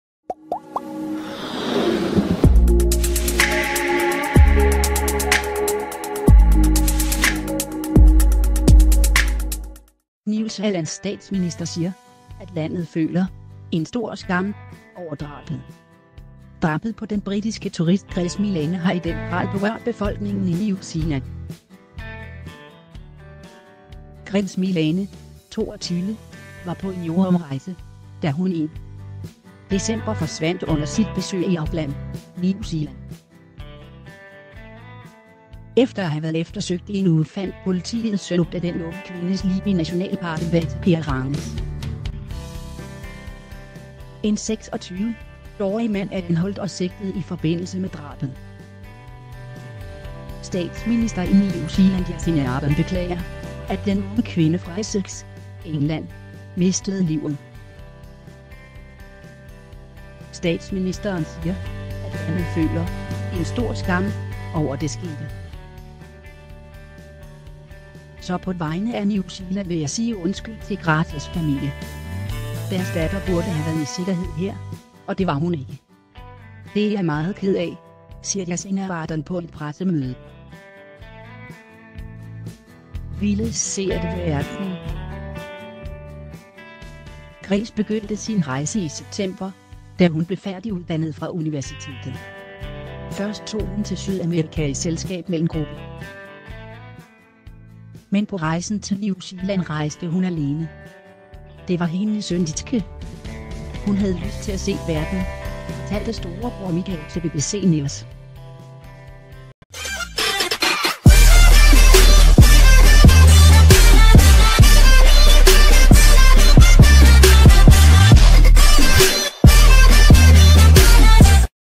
✅ Grace dræbt på drømmerejsen: Berørt statsminister undskylder
✍ New Zealands statsminister siger, at landet føler 'en stor skam' over drabet. Drabet på den britiske turist Grace Millane har i den grad berørt befolkningen i New Zealand.. Grace Millane, 22, var på en jordomrejse, da hun 1. december forsvandt under sit besøg i Auckland, New Zealand. . Efter at have været eftersøgt i en uge fandt politiet søndag den unge kvindes lig i nationalparken Waitakere Ranges.. En 26-årig mand er anholdt og sigtet i forbindelse med drabet. . Statsminister i New Zealand Jacinda Ardern beklager, at den unge kvinde fra Essex, England, mistede livet. . Statsministeren siger, at landet føler 'en stor skam' over det skete.. Så på vegne af New Zealand vil jeg sige undskyld til Graces familie. Deres datter burde have været i sikkerhed her, og det var hun ikke. Det er jeg meget ked af, siger Jacinda Ardern på et pressemøde.. Ville se verden. Grace begyndte sin rejse i september, da hun blev færdiguddannet fra universitetet. Først tog hun til Sydamerika i selskab med en g ...